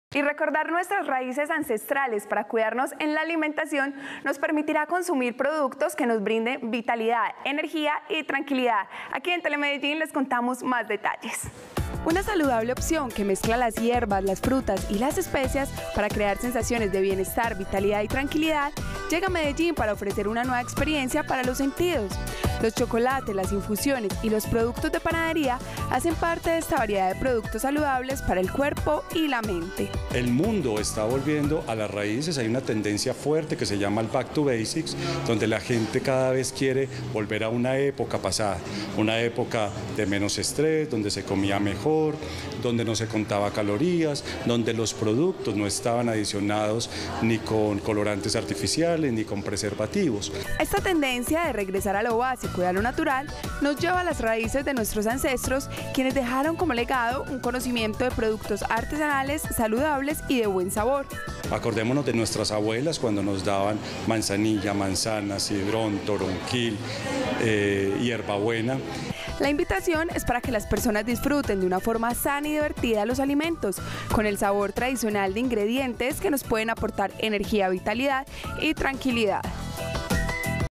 chocolates, infusiones y productos de panadería crean sensaciones [Noticias] - Telemedellín
Una forma diferente, divertida y saludable de comer chocolates y parva, es posible en nuestra ciudad. Un centro de experiencias en donde la comida nos despierta todos los sentidos